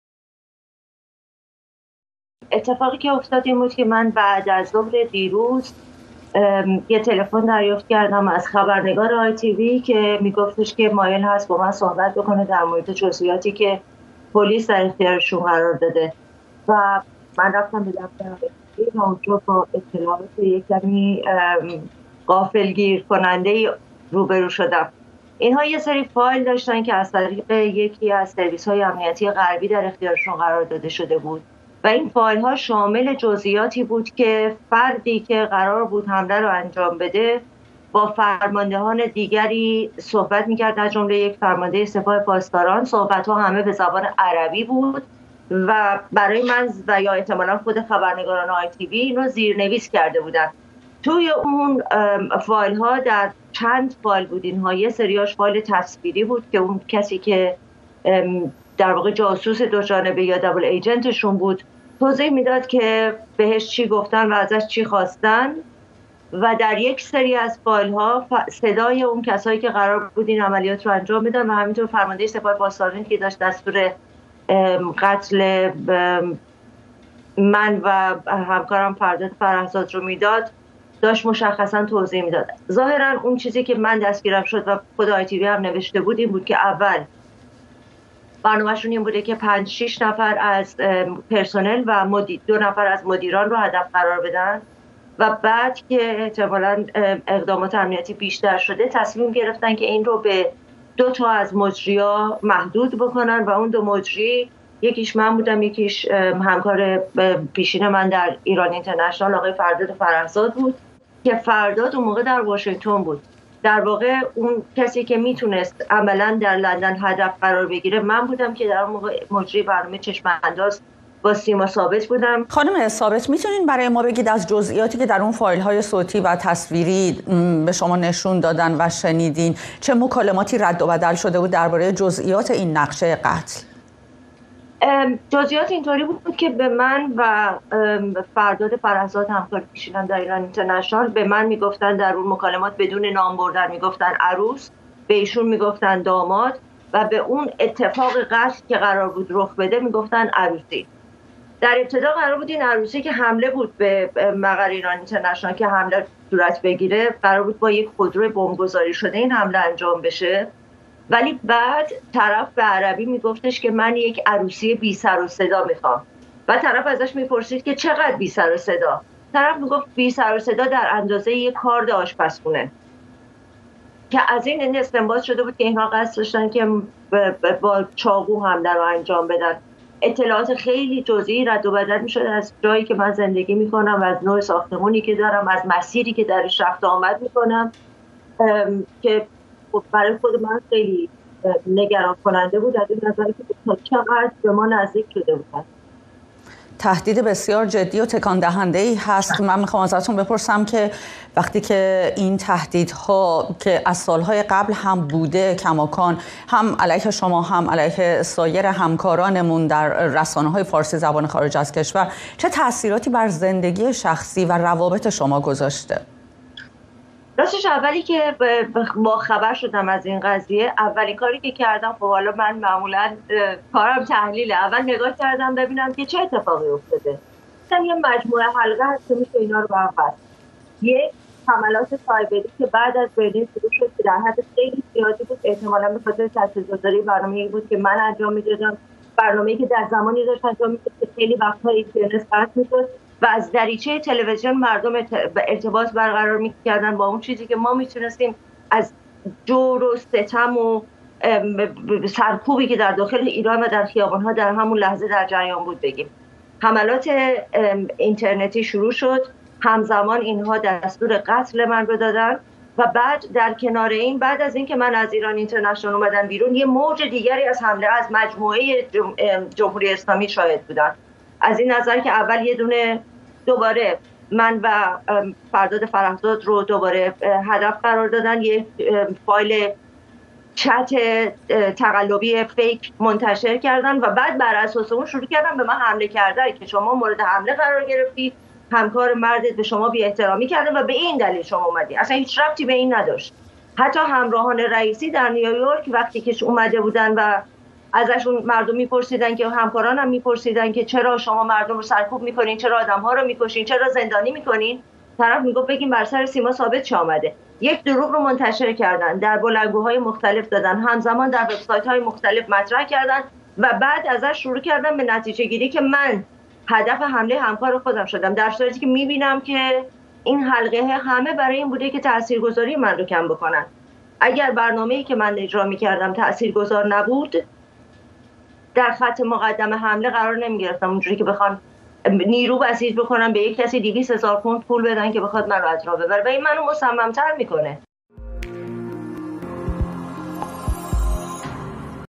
افشای طرح ترور سیما ثابت توسط جمهوری
نخستین گفتگوی سیما ثابت پس از افشای طرح ترور او توسط جمهوری